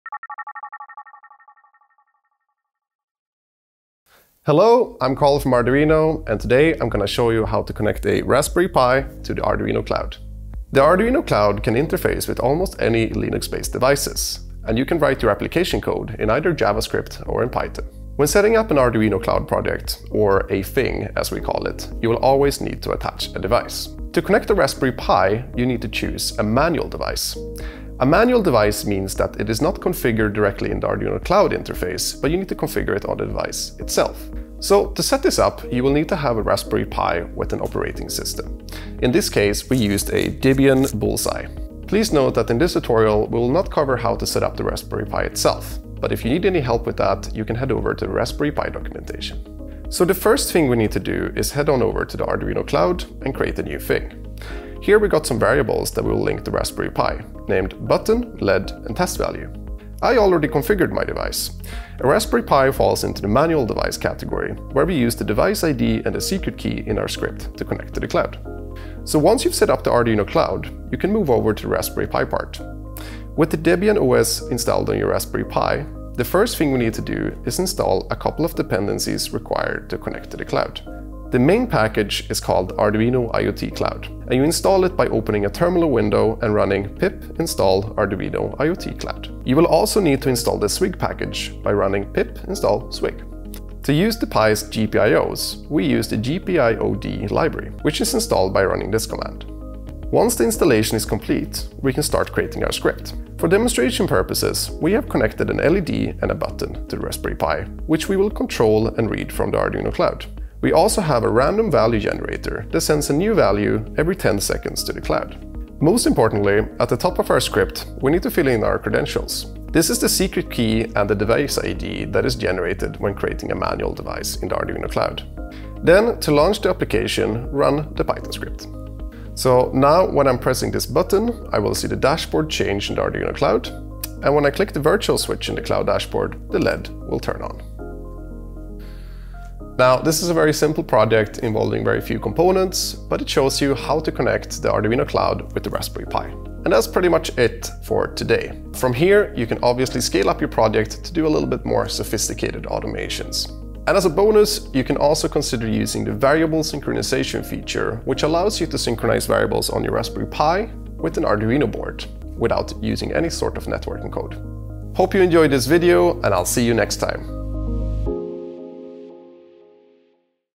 How to Send Your Raspberry Pi Data to Arduino Cloud
Raspberry Pi and other Linux-based platforms have become popular in the IoT space due to their versatility and ease of use. However, a common issue is finding an effective way to visualize their data and connect them to visualization platforms.

Connecting your Raspberry Pi to the Arduino Cloud cannot be easier. You only have to follow these steps:

1. Set up an Arduino Cloud account.
2. Add your device to the Arduino Cloud as a Manual device.
3. Create your Thing and add your variables.
4. Create the code for your Raspberry Pi using your preferred development language and environment.
5. Create the dashboards for data visualization.

Control your Raspberry Pi GPIOs from Arduino Cloud using Python in this project